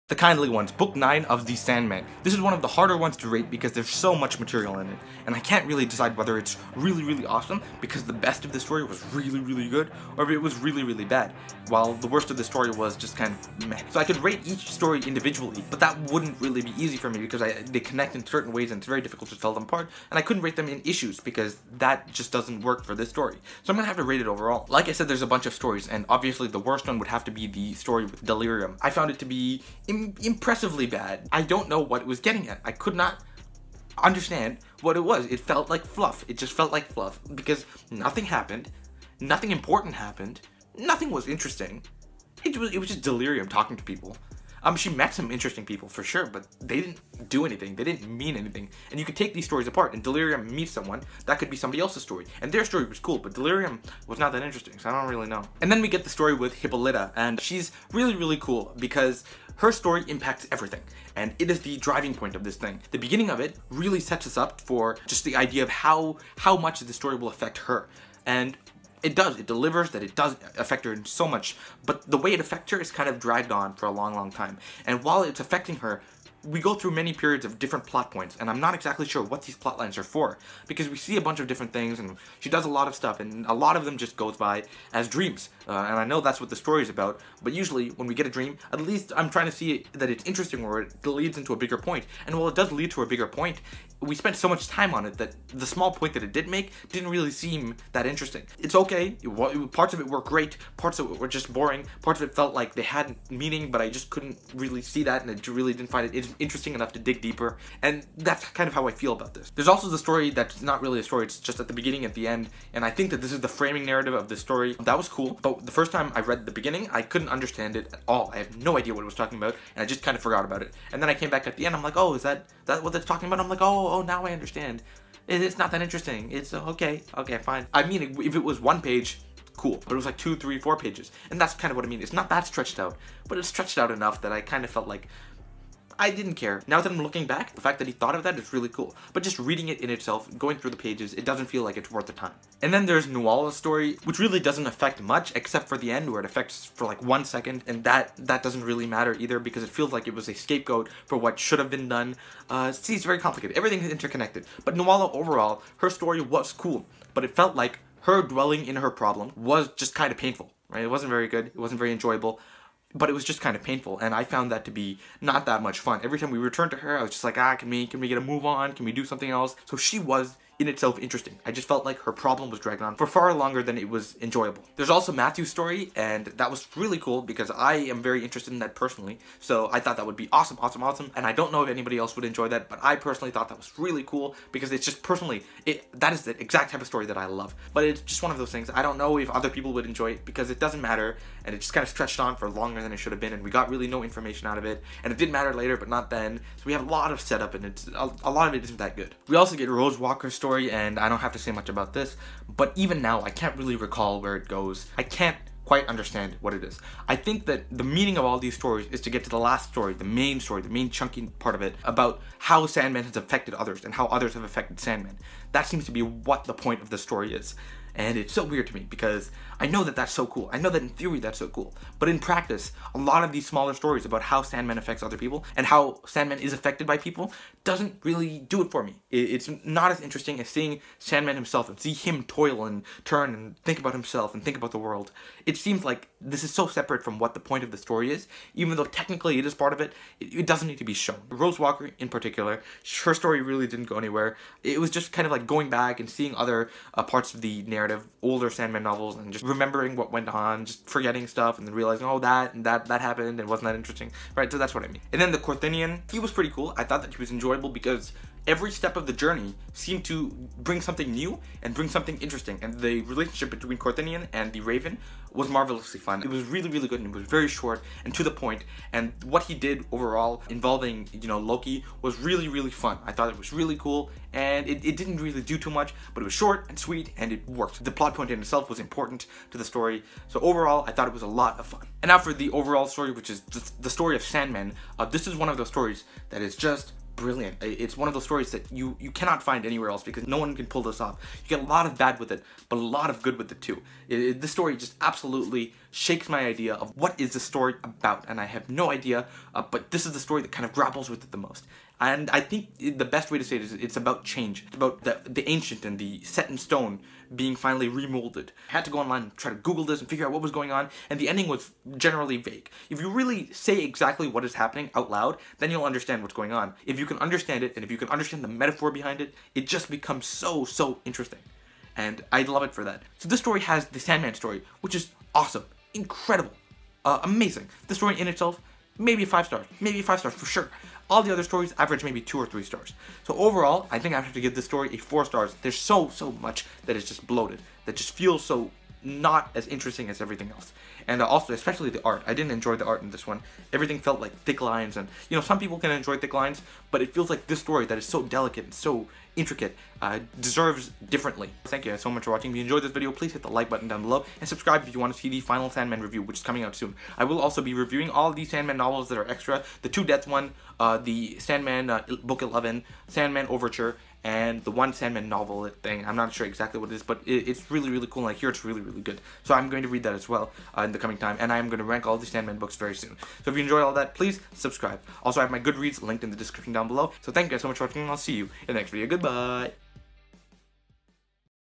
The Kindly Ones (The Sandman #9) REVIEW
Book: The Sandman: The Kindly Ones

Thoughts: Just mind bogglingly interesting. The peak of sandman, undoubtedly, despite the massive amount of stuff that happened that barely mattered.

Rating: 4/5